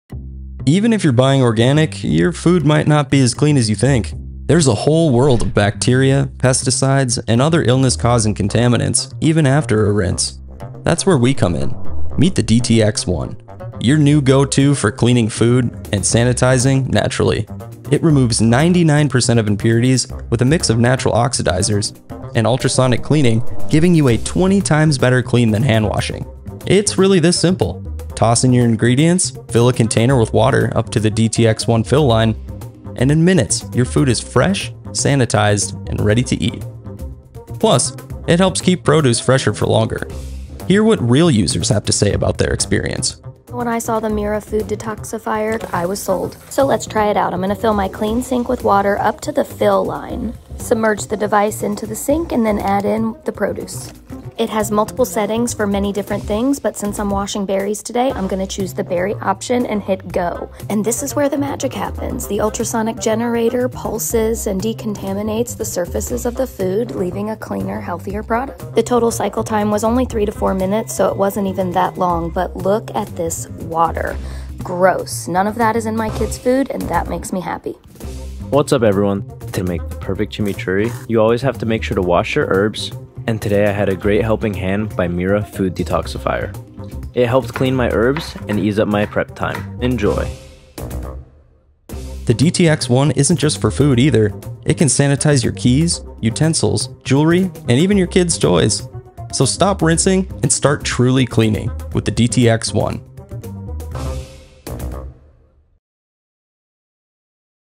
MIRA Safety DTX-1: Revolutionize Food Safety with This Oxidizing Detoxifier!
Chemical threats don’t just exist on the battlefield—they start at home with the food we consume.

MIRA Safety, a trusted leader in CBRN PPE, introduces the DTX-1 Oxidizing Food Detoxifier, a cutting-edge solution to eliminate harmful pesticides, bacteria, and contaminants from meat, fruits, and vegetables. This revolutionary device extends MIRA Safety’s mission of protecting you from chemical threats to your kitchen, ensuring every bite is as safe as the gear you rely on.